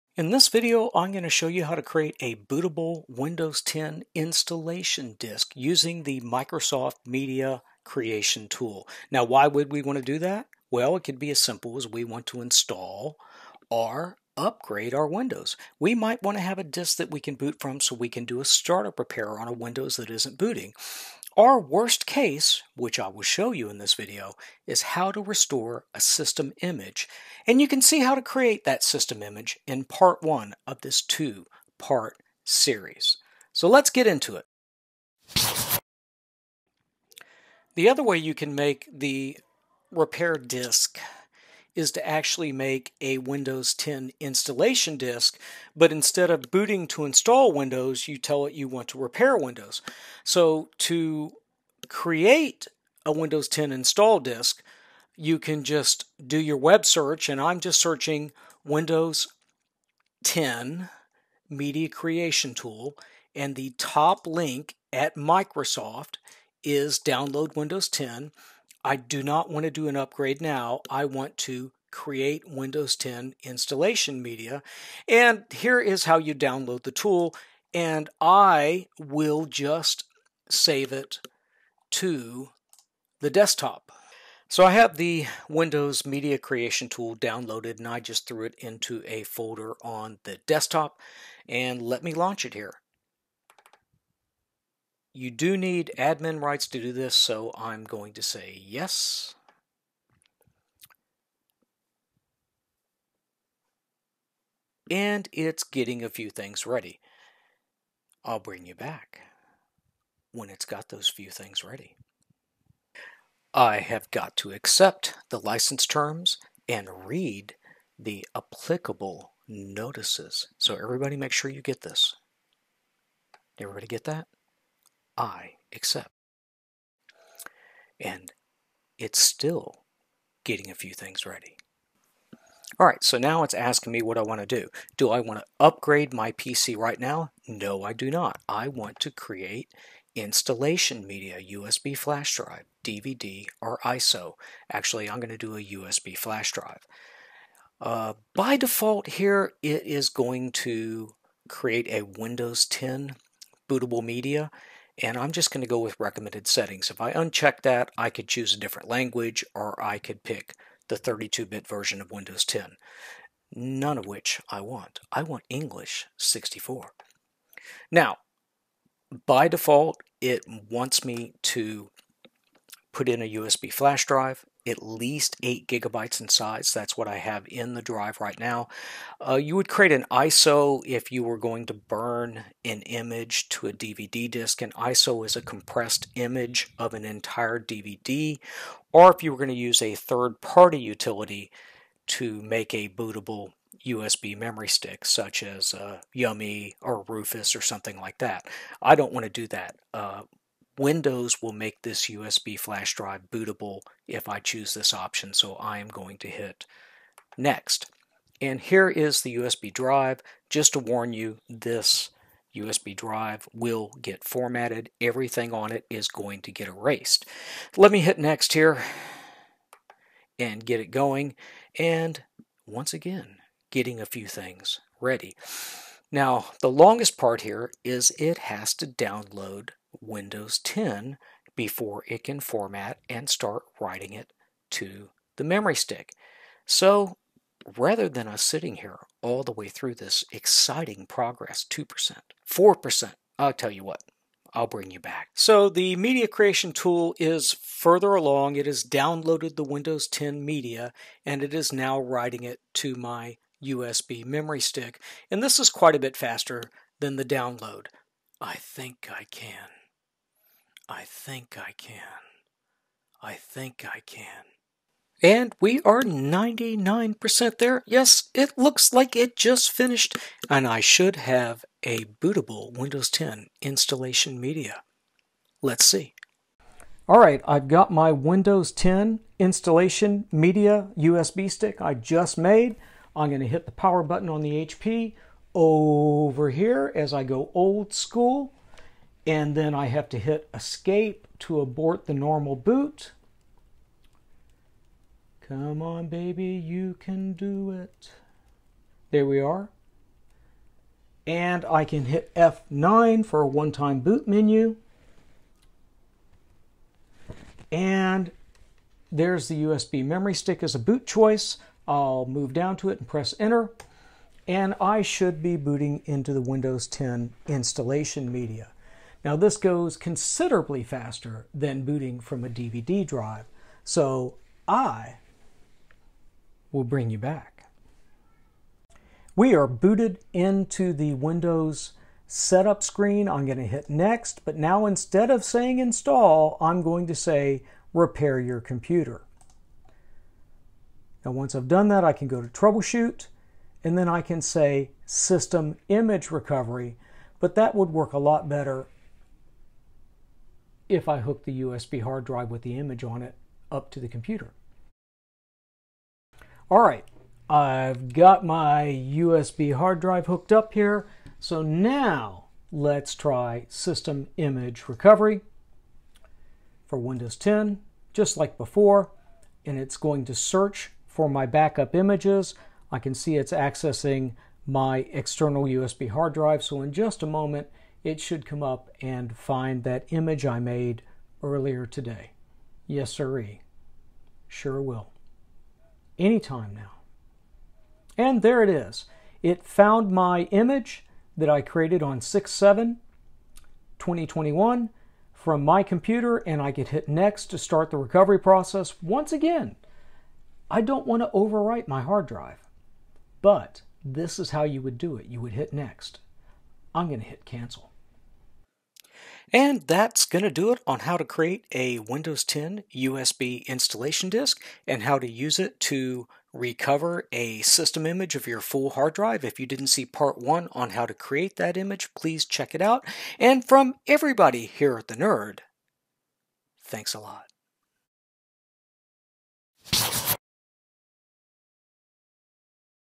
Windows 10 Install USB
Create a bootable USB to install Windows 10, repair your PC, or recover a full image backup to a hard drive. This is part 2 of creating and restoring a Windows 10 System Image.
00:01 Intro
00:37 Create a bootable Windows 10 install USB drive
04:37 Booting from the Windows 10 USB stick
05:30 Recovering your Backup Full System Image to your PC
06:48 Outro